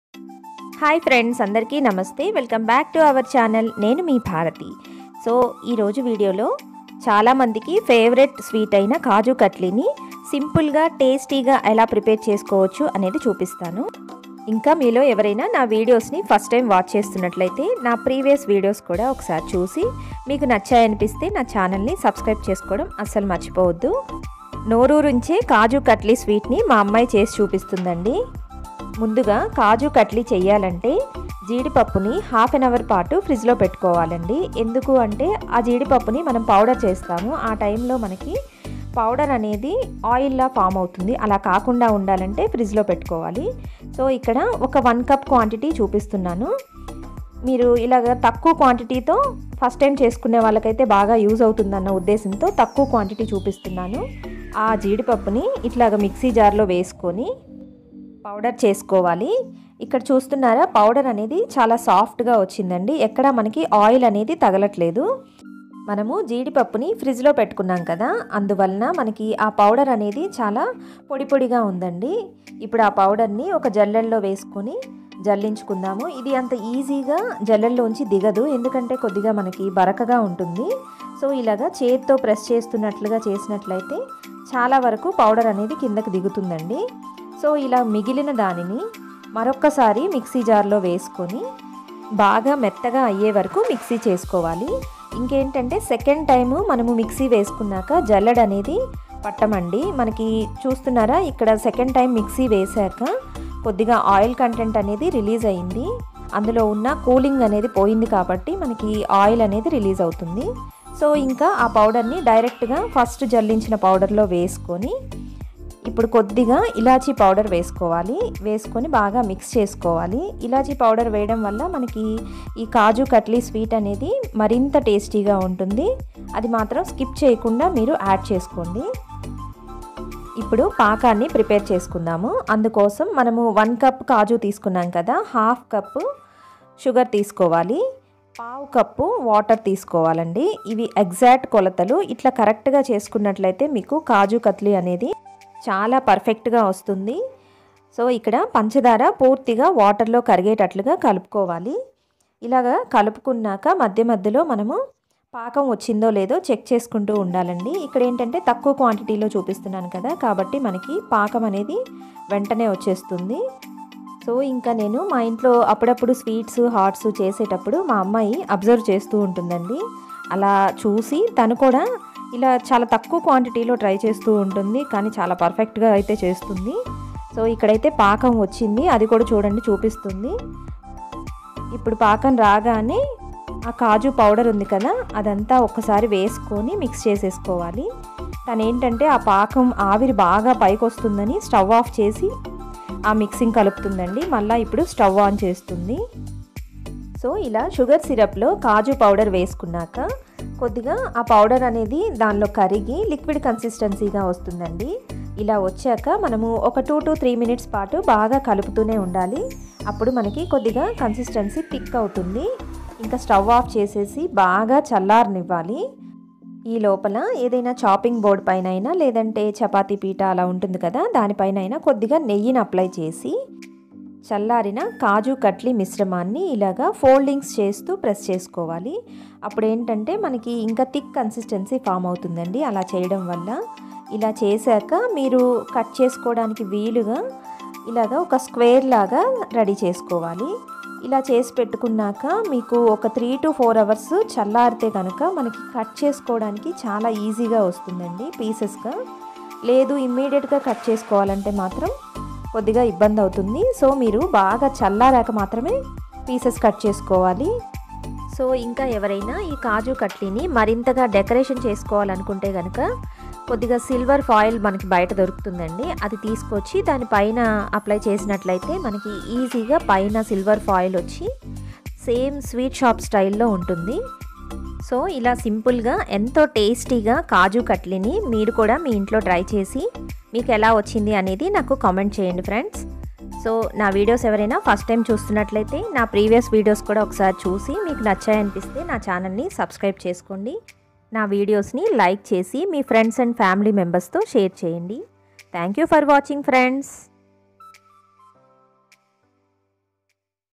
Super tasty kaju katli||Sweet shop style kaju katli with tips||Home made kaju barfi
hi friends today I am going to show you kaju katli in a very simple way you can make this at home very easily so please try this recipe and let me know your opinion